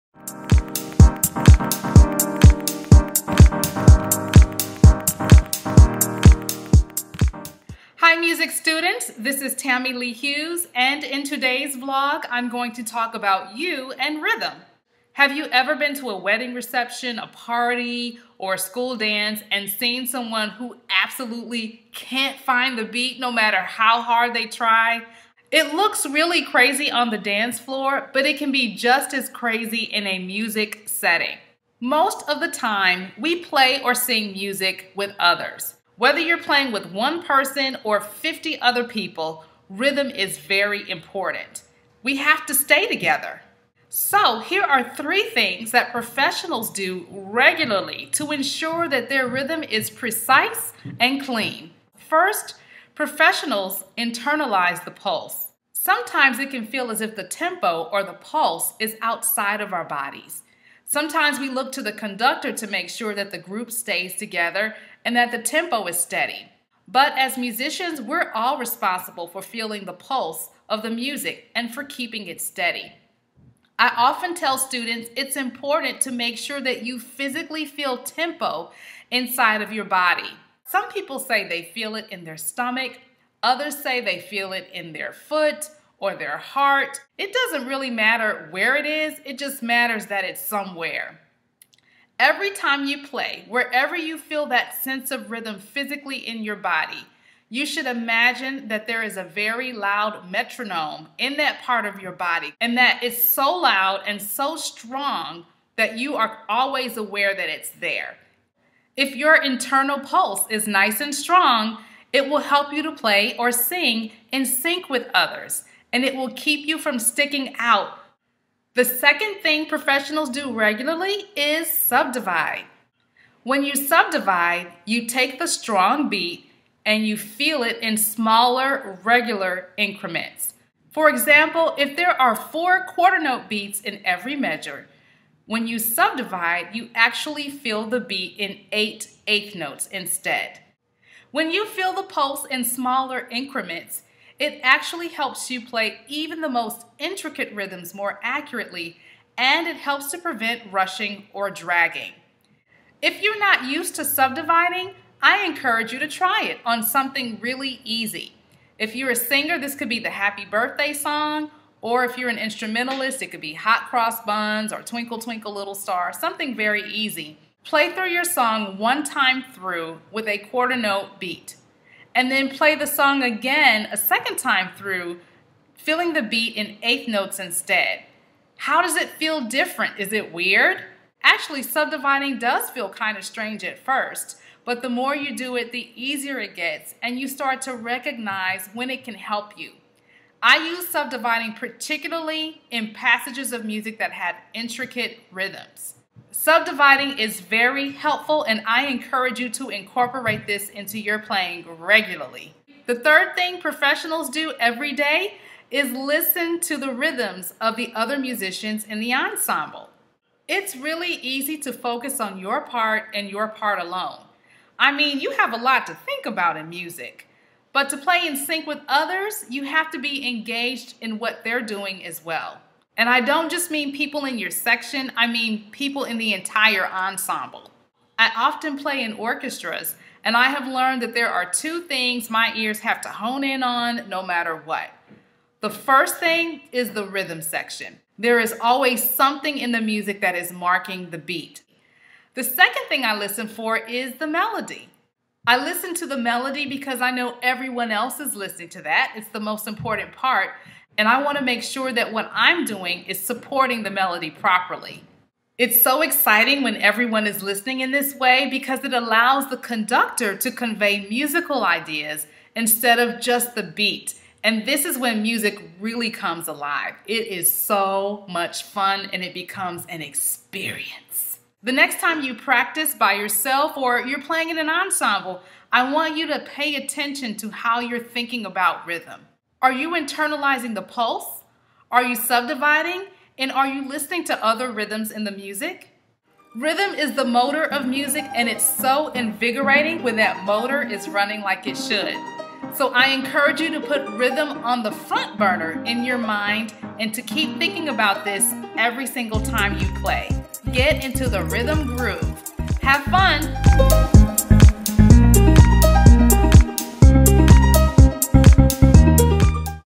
VFMS: Rhythm and You - Think Like the Pros
Concert Violinist and Educator Tami Lee Hughes shares three techniques music students should master to ensure their rhythm is as clean and precise as that of professional artists.